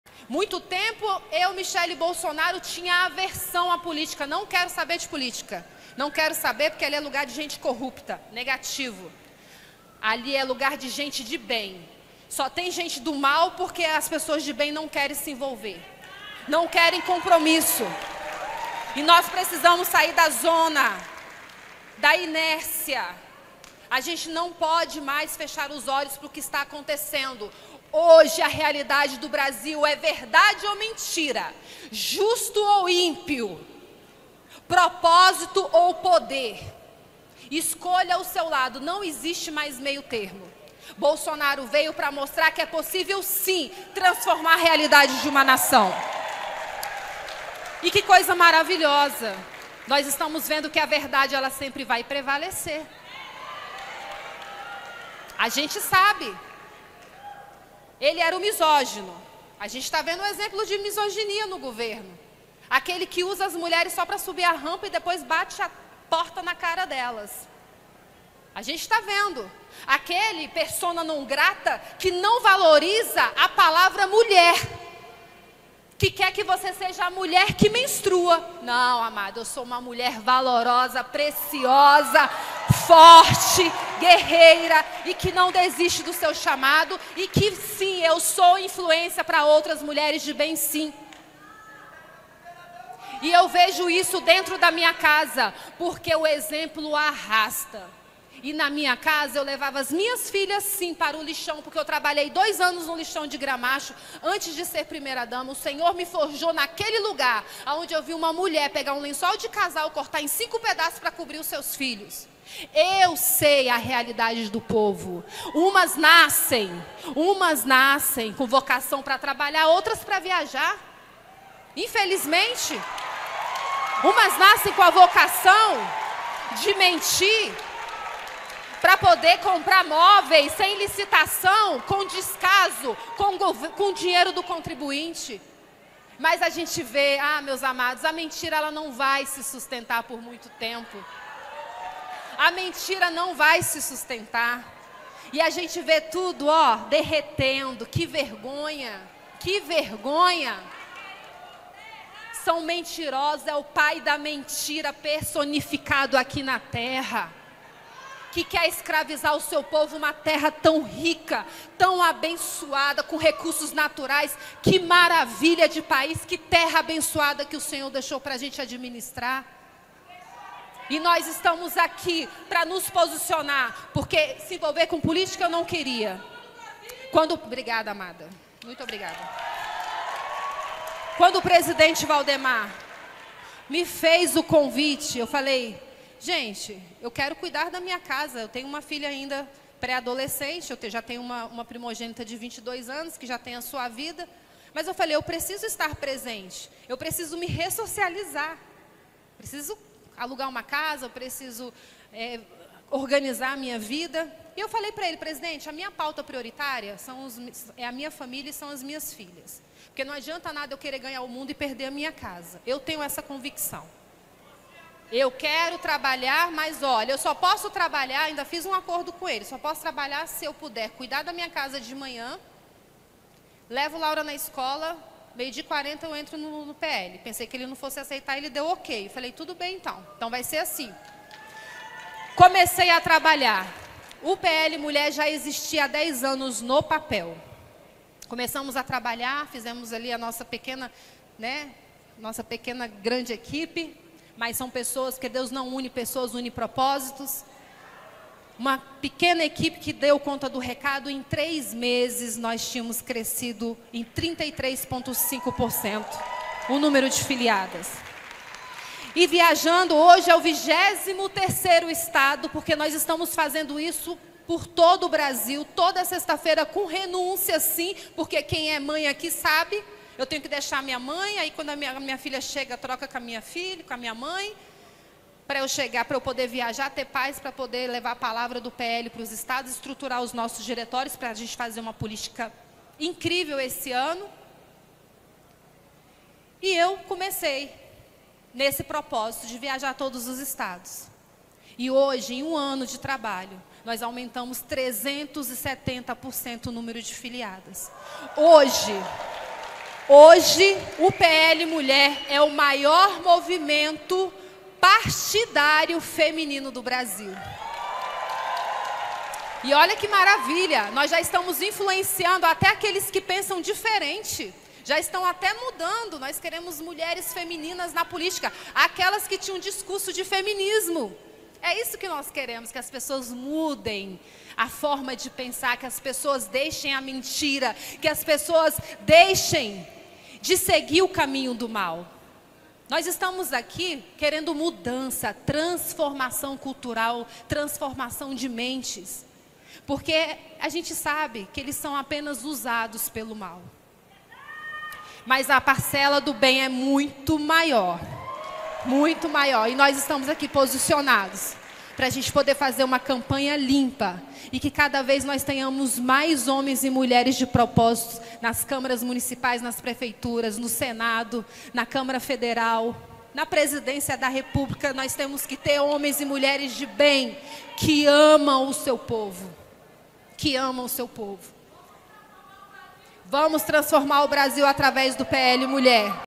MICHELLE MANDA INDIRETA PARA JANJA: "UMAS NASCEM COM VOCAÇÃO PARA TRABALHAR OUTRAS PARA VIAJAR"
A ex-primeira-dama do Brasil, Michelle Bolsonaro, mandou uma indireta para a atual primeira-dama, Janja da Silva durante discurso neste sábado (04). Michelle afirmou que enquanto "umas nasceram para trabalhar, outras nasceram para viajar".